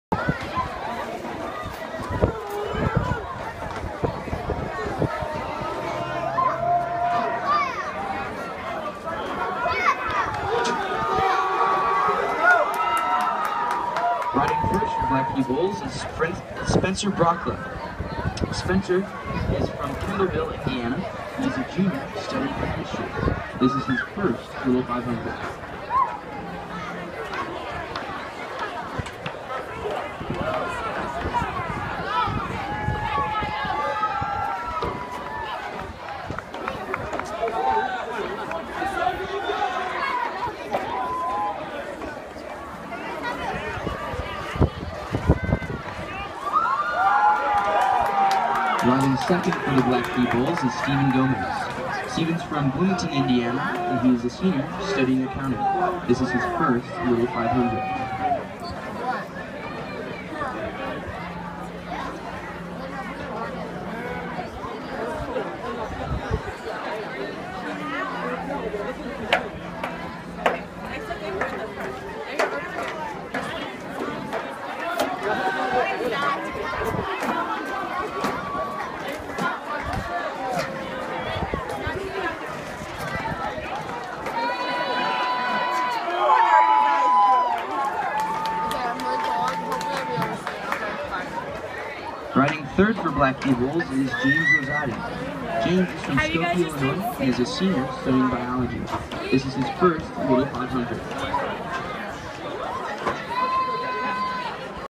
Black Key Bulls 2014 Little 500 Qualifications (First Half)
BKB Qualified for the 64th running of the Men's Little 500 with a time of 2:23.65.  Unfortunately, our pledges ran out of space on the memory card.